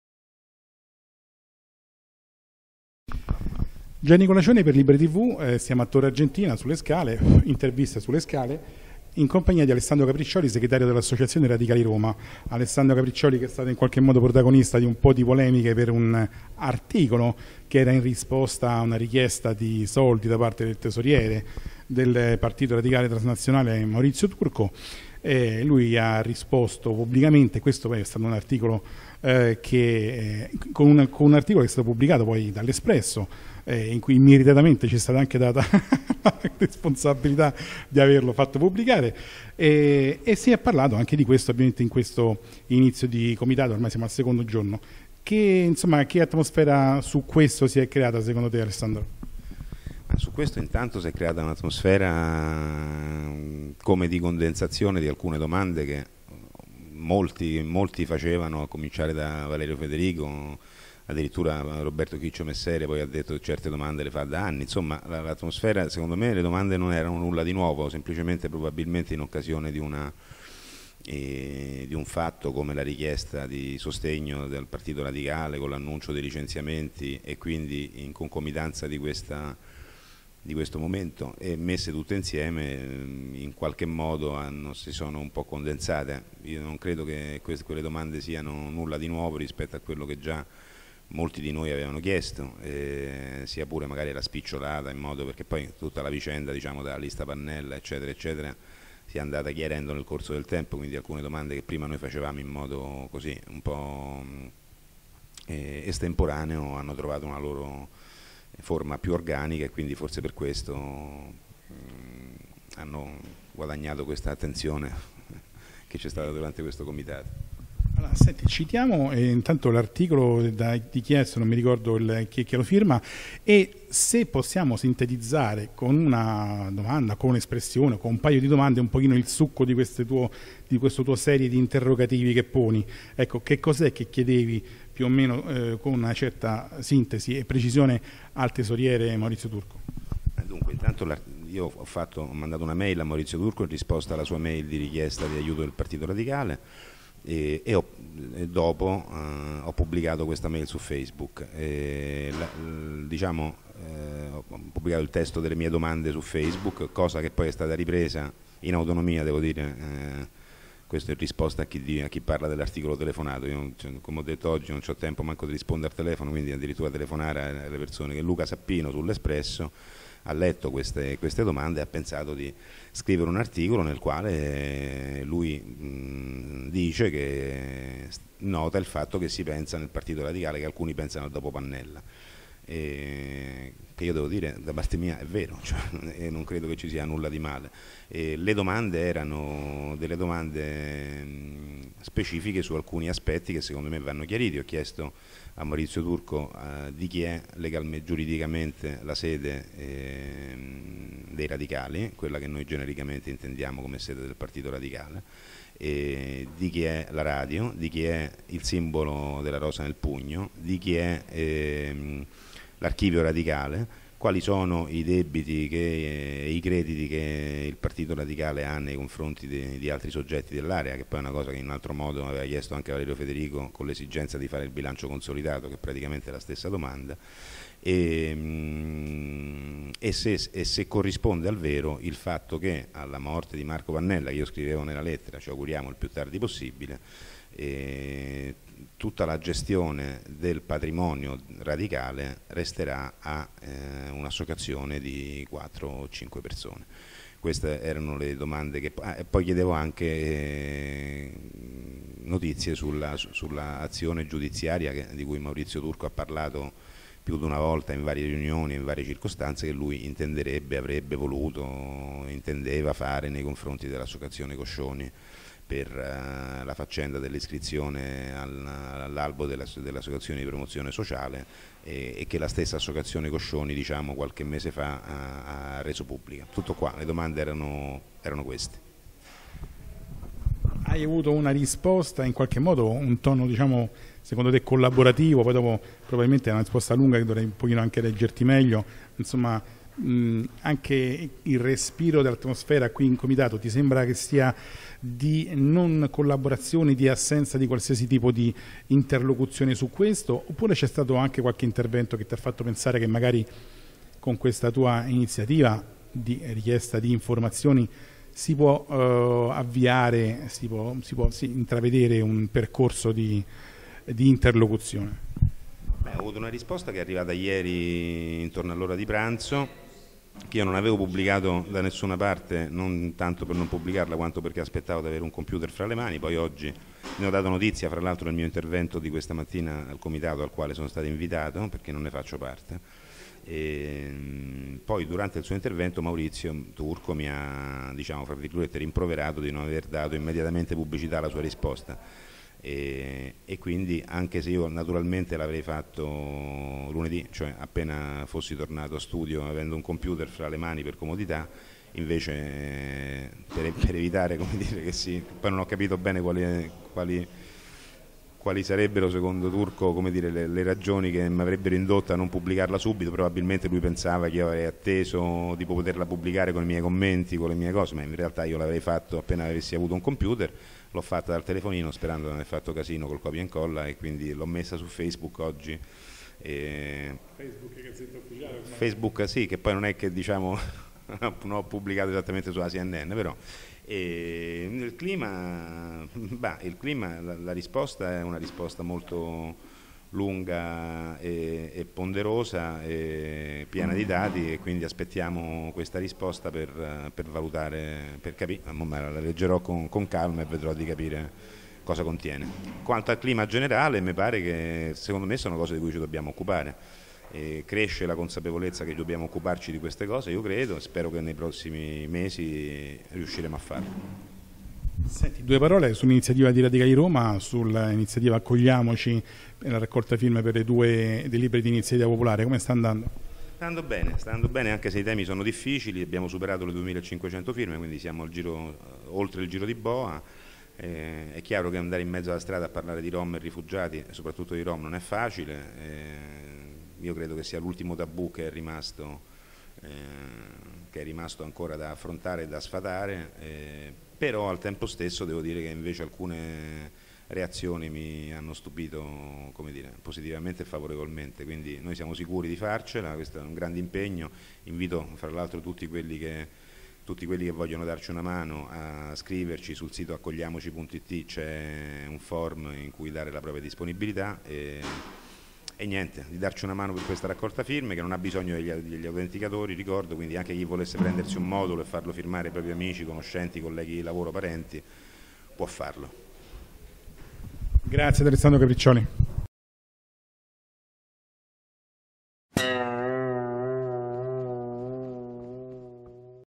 Alessandro Capriccioli e ...l'articolo telefonato
Intervista ad ALESSANDRO CAPRICCIOLI, Segretario dell'associazione Radicali Roma a margine del Comitato Nazionale di Radicali Italiani